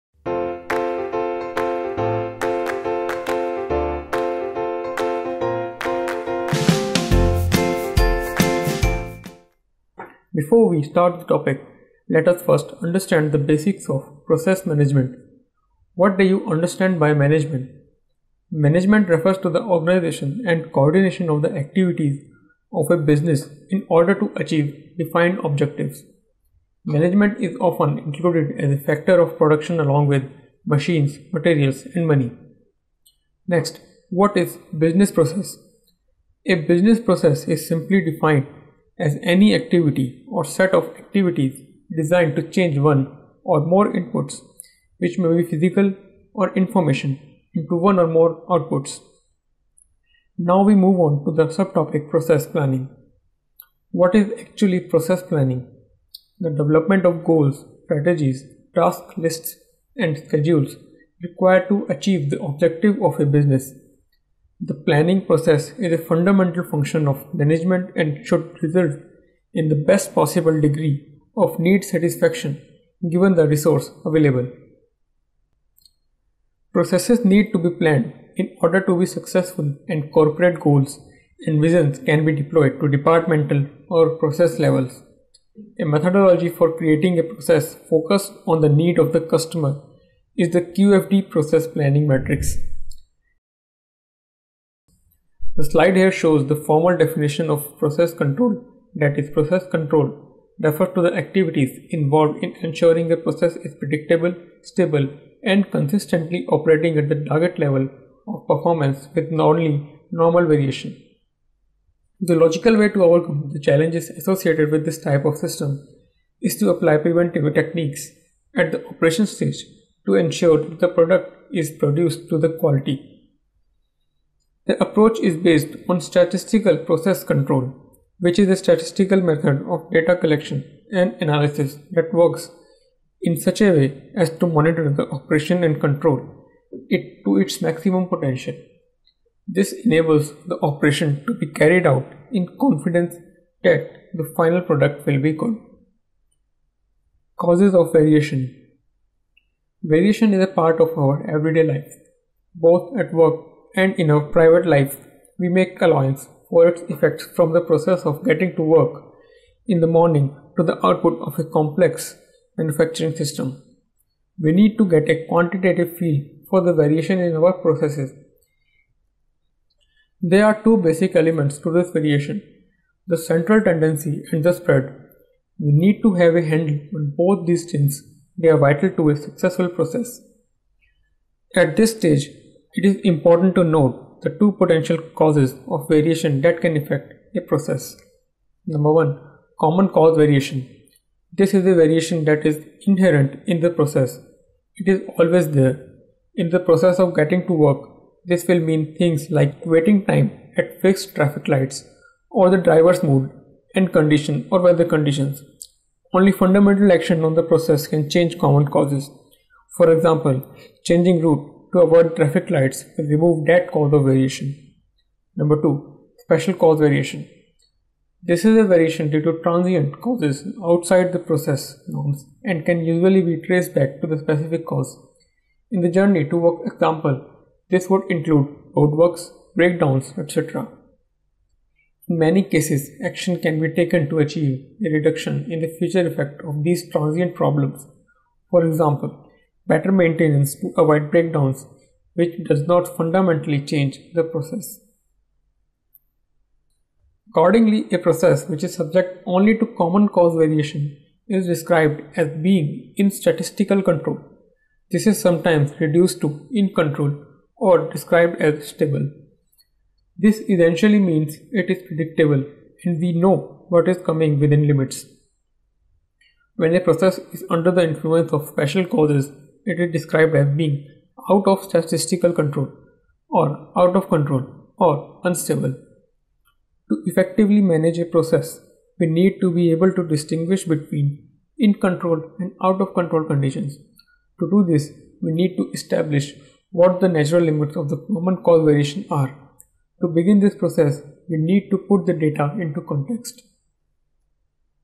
Total Quality Management Tutorial | Process Management - Chapter 10
This Vskills tutorial of Quality on Total Quality Management covers topics of Process Management & Process Control